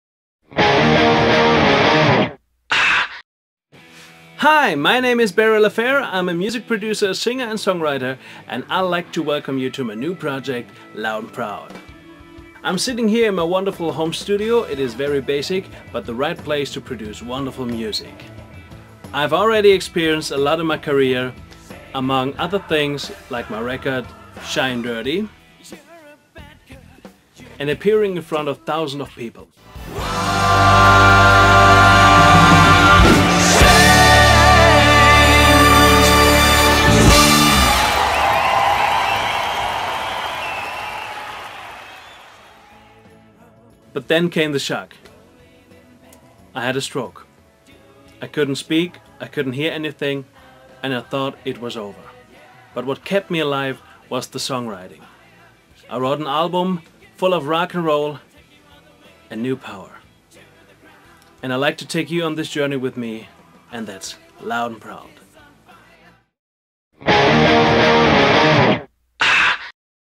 INTRODUCING BARRY L'AFFAIR LOUD'N PROUD
INTRODUCING BARRY L’AFFAIR A KID WITH RAW TALENT
Introducing Barry L’Affair a kid with raw talent, great looks, a voice George Michael would quit spliff for and a songbook of finally crafted, funking, rocking hits-in-waiting.Singer, songwriter, producer a German TAFKAP, exploding with music. His own man. His own groove. 25 years young and already working with Grammy winning producers and songwriters like Mark Hudson / the team of Aerosmith, Bon Jovi, Prince, Michael Jackson, Celine Dion, Paul McCartney, Ozzy Osbourne and many more... Barry has been honing his talent since age three. At 12, he was gigging across Europe, from Ibiza to the Norfolk Broads. Today, he has 10 albums worth of songs ready to hit the stage and a constant flow of new material in makeup. He lives and sleeps in his studio. Just him, all the names of the music world’s biggest musicians behind the glass, a select group of session players …and an 80-piece orchestra. Barry can wake up with an idea and have a demo down before his first coffee. Barry gets into musical collaborations like other people get into jackets. Barry’s music rocks. And funks. So stay in music and be funky